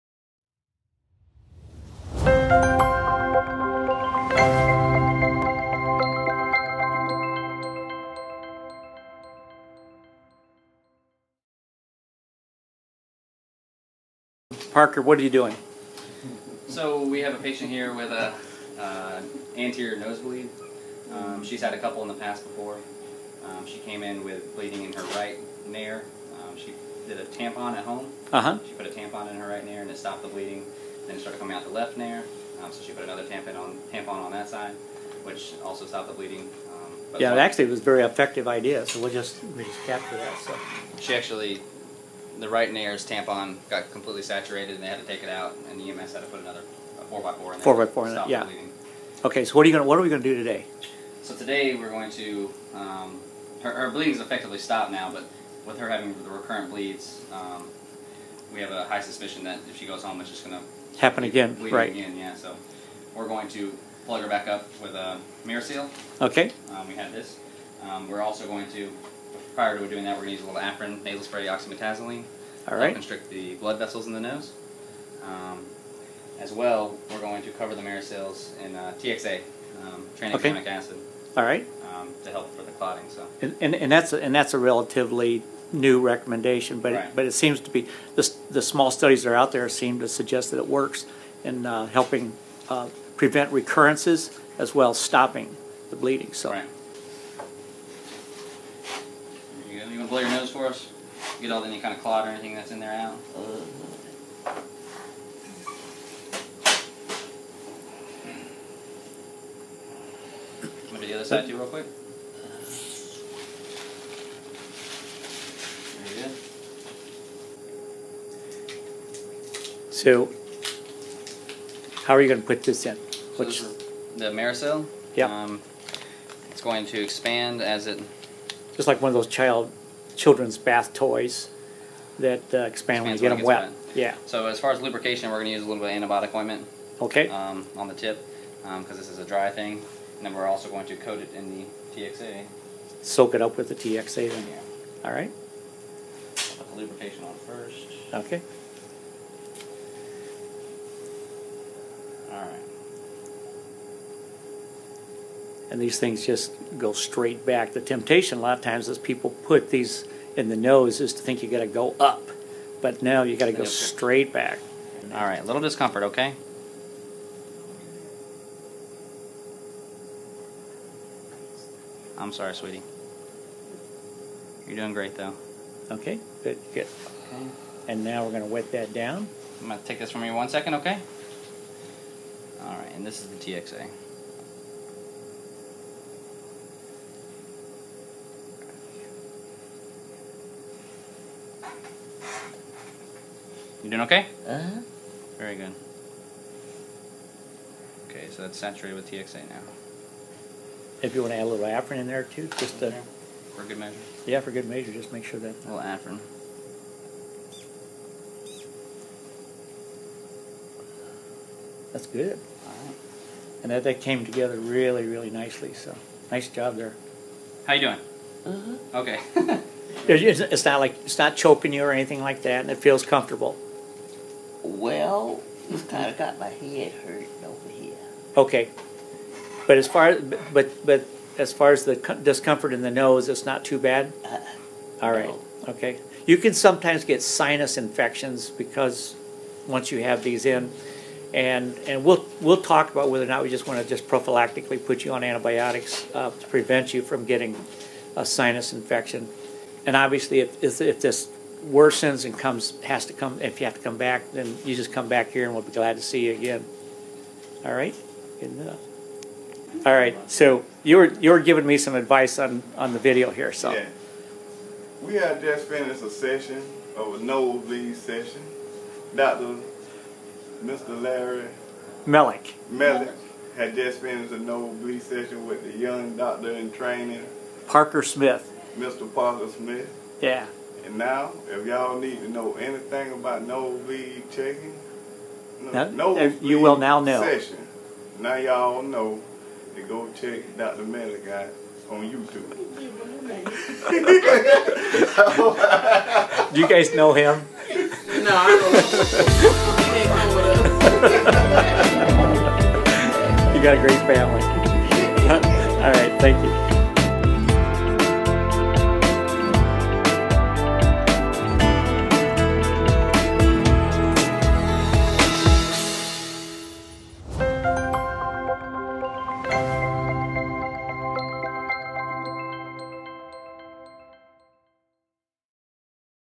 Nosebleed Emergency and Tranexamic Acid Treatment
In this video we demonstrate how to use a nasal tampon soaked with tranexamic acid to stop a nosebleed.  This video was created at the Medical College of Georgia emergency department.